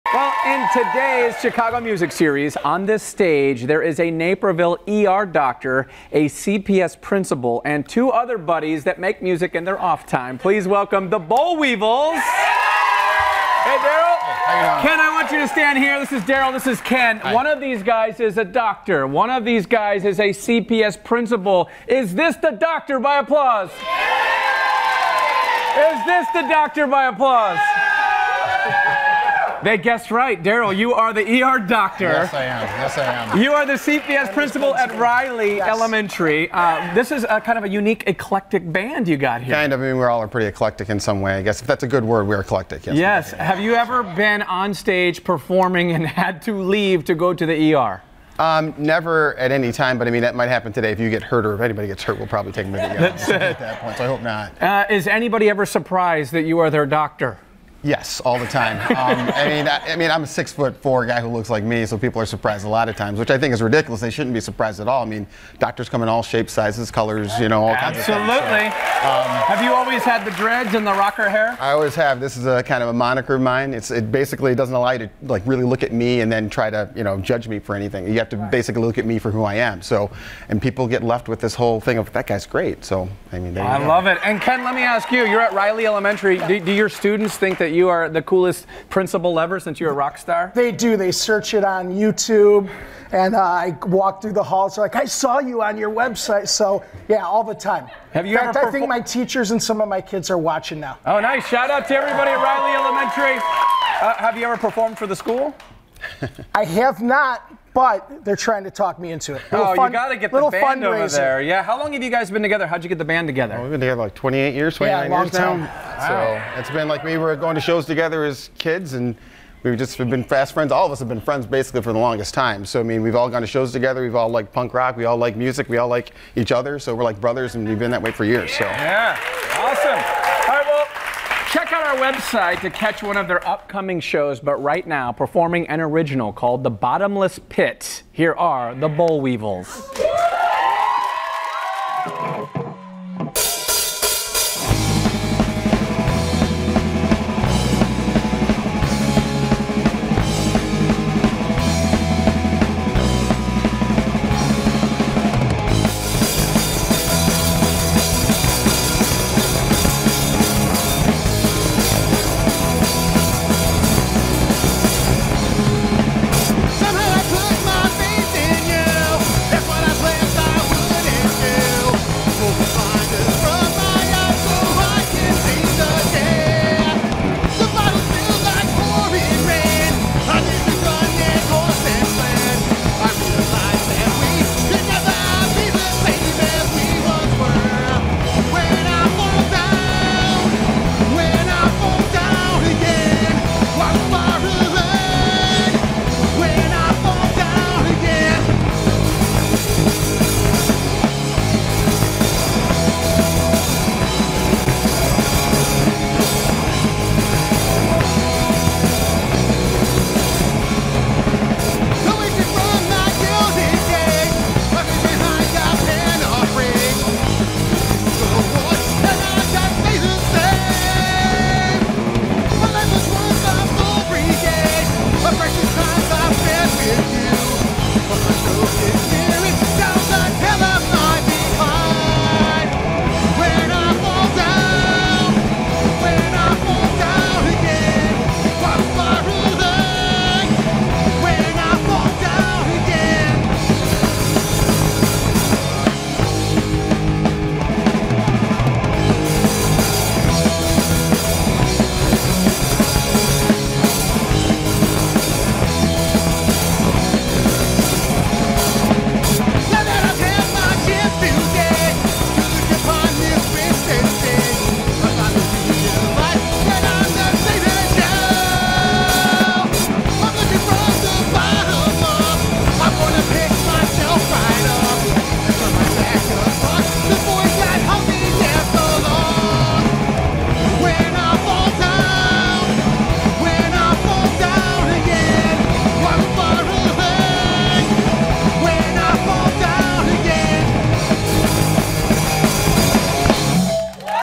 Punk band The Bollweevils perform
Naperville dad Dr. Daryl Wilson runs an ER by day and fronts a punk rock band by night.